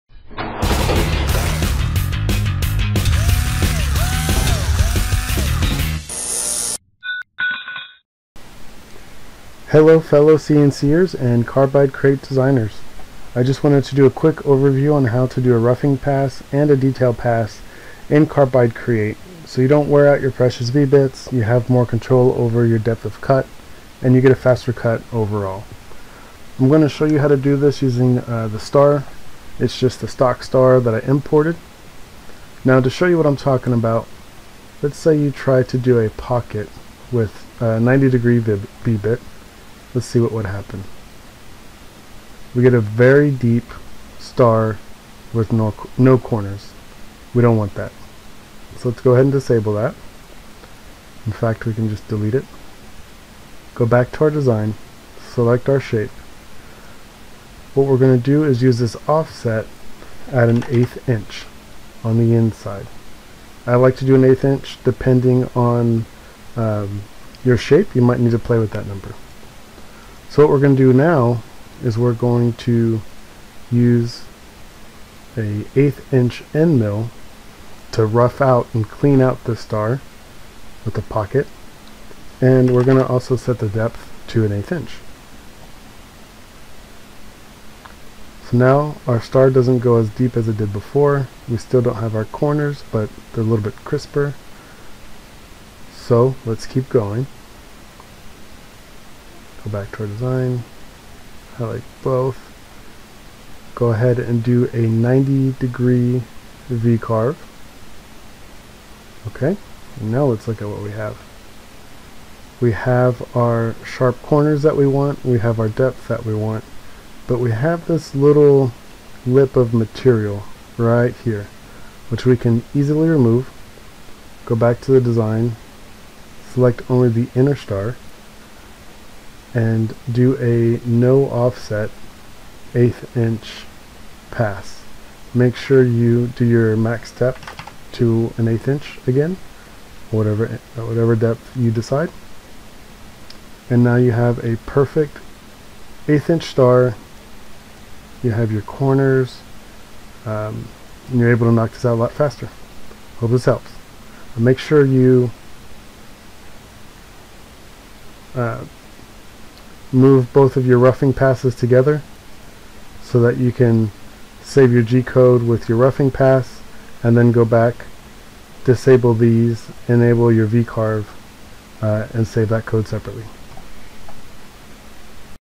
Carbide Create Roughing and Detail Pass
When working with carbide create and vcarves, it can be annoying to not have the ability to set your depth of cut or speed up your cuts by using a roughing pass first. This is how to do just that, much like you can do in Easel.